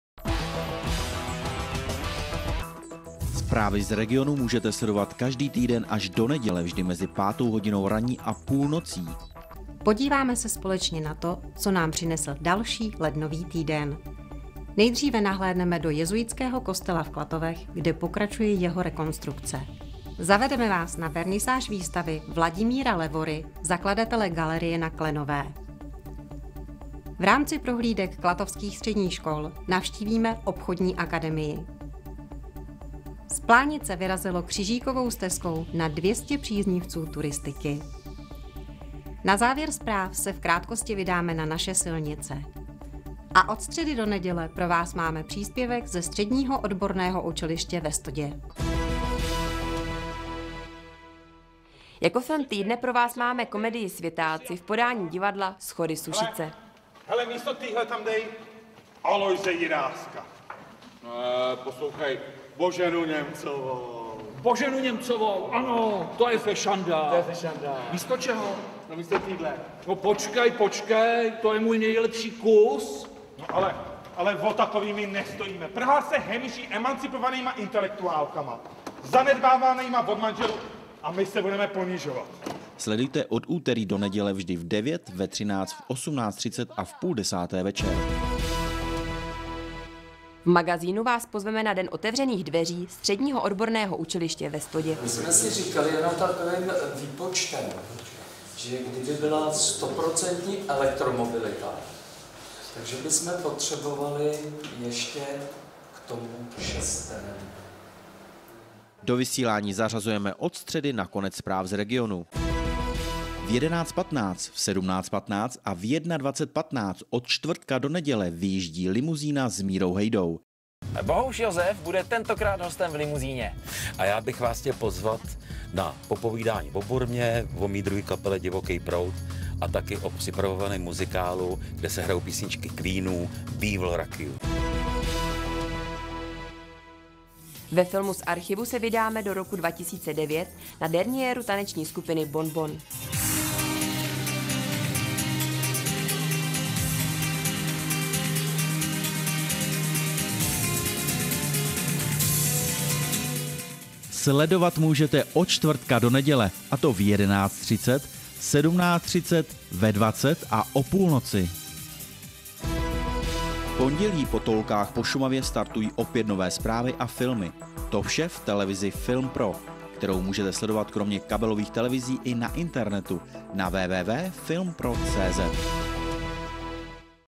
Program televize FILMpro 20. -26.1. 2020
regionální televize Plzeň-jih * Klatovsko * Šumava

PONDĚLÍ-NEDĚLE  5:00-24:00
Zprávy z regionu
Rekonstrukce jezuitského kostela v Klatovech
Výstava obrazů Vladimíra Levory
Obchodní akademie Klatovy
Křižíkova stezka z Plánice
Dopravní nehody na Plzeňsku

ÚTERÝ-NEDĚLE 9:00, 13:00, 18:30 a 21:30
Film týdne:  Světáci (Schody Sušice) 1/3 (délka: 34 min.)

STŘEDA -NEDĚLE (po zprávách z regionu)
Den otevřených dveří SOU ve Stodě

ČTVRTEK-NEDĚLE
11:30, 17:30, 20:00 a 24:00 Film z archivu: Bonbon - derniéra 2009 2/4 (20 min.)
11:15, 17:15 a 21:15 Limuzína s Mírou Hejdou: Bohouš Josef (15 min.)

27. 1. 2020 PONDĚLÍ
5:00 Toulky po Šumavě - pásmo krátkých filmů
odpoledne premiéra nových Zpráv z regionu